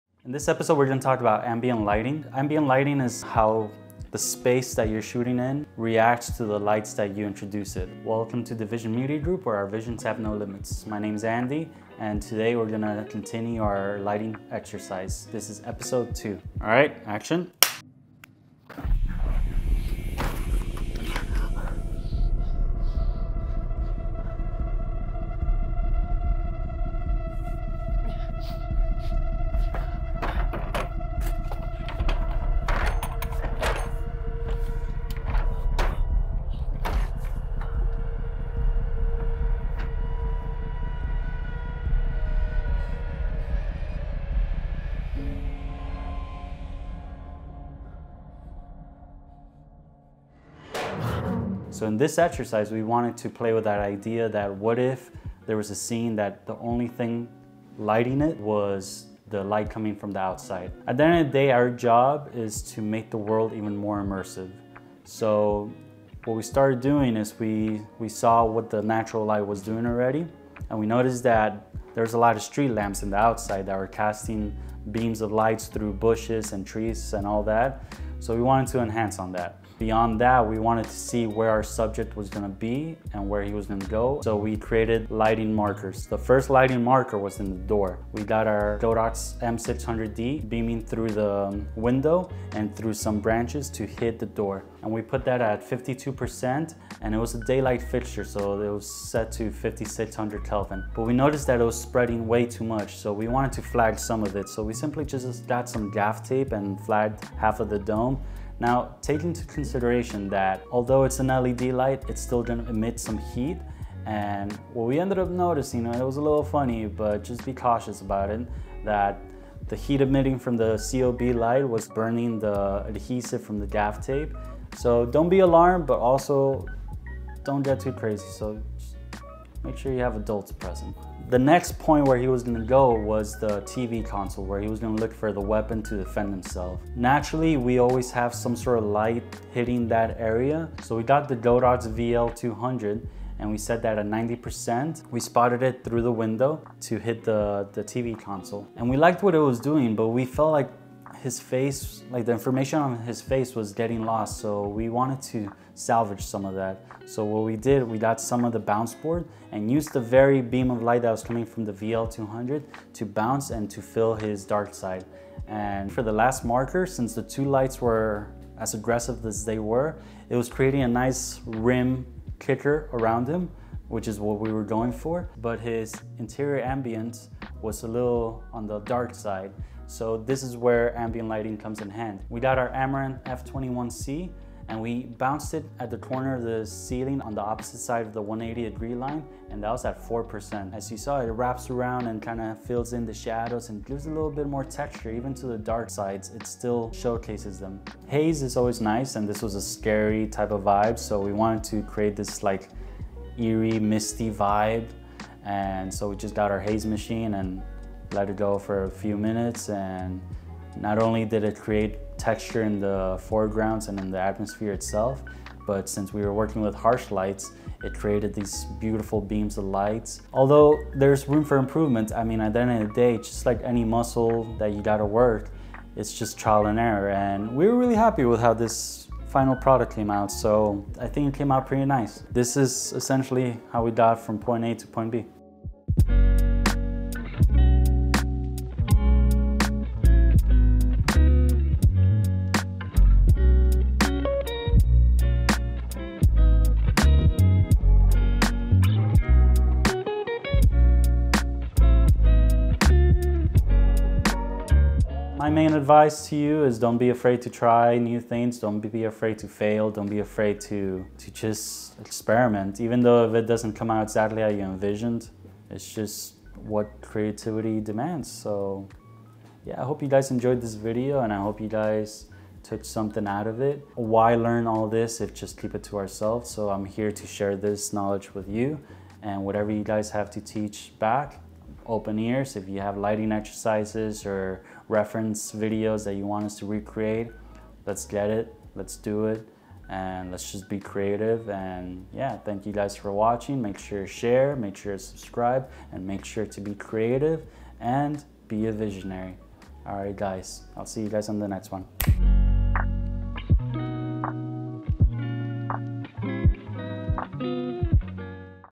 Ambient Lighting | Lighting Tutorial EP. 02
In this video we will be approaching our lighting setup in the assumption of all the light coming from the outside in and how it would behave from within.

To see the BEFORE & AFTER + our Lighting Diagram, make sure to visit our instagram page listed below along with the creators that were involved in the making of this video!

CREATORS
Andy Sarmiento

Gear Used
-Godox M600D
-Amaran f21c
-Godox VL200
-BMPCC 6K Pro
-Sigma 18-35 Art